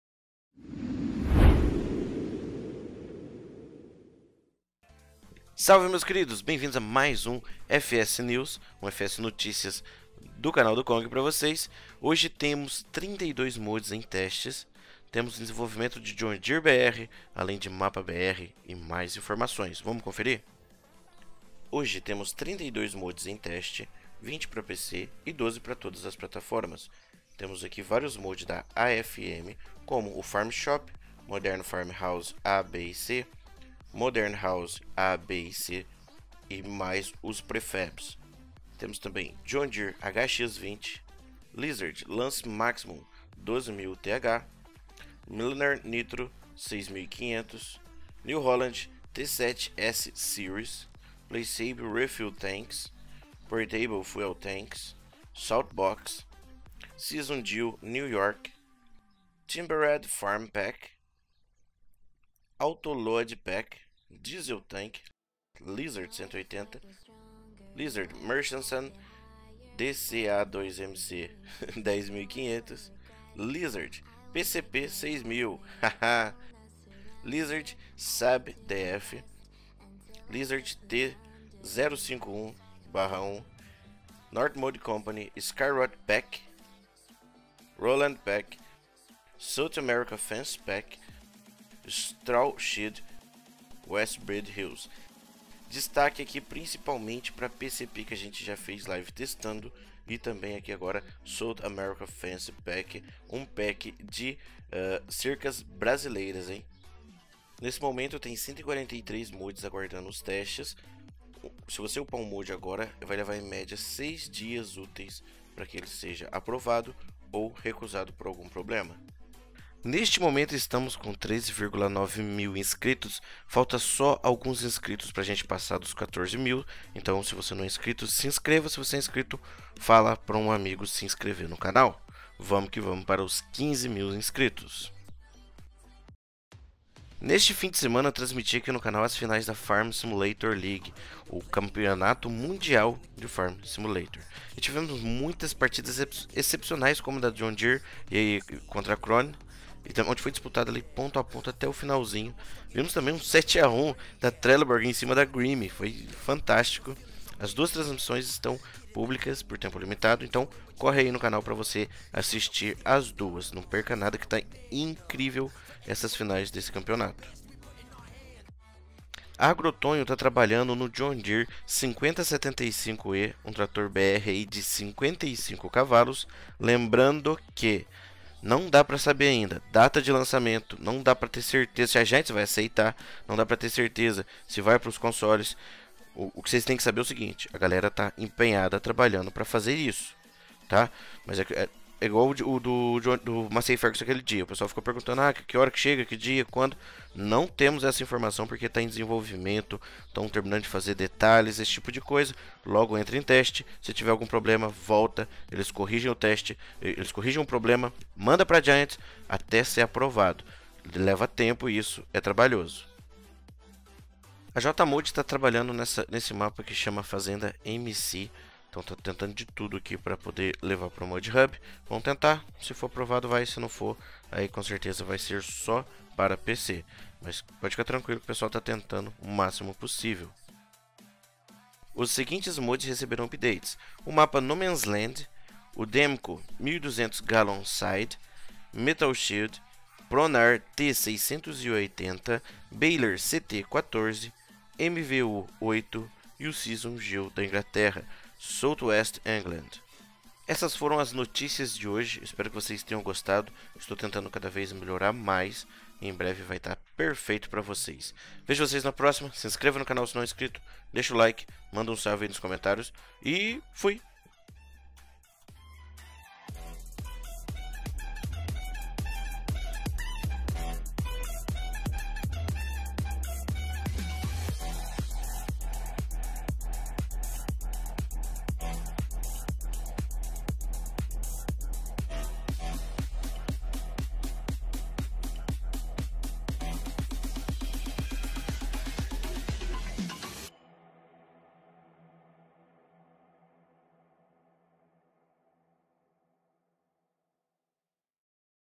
NOTÍCIAS | John Deere Brasileiro, Mods em testes, Atualizações e mais | FS NEWS #2
Notícias fresquinhas do mundo Farming Simulator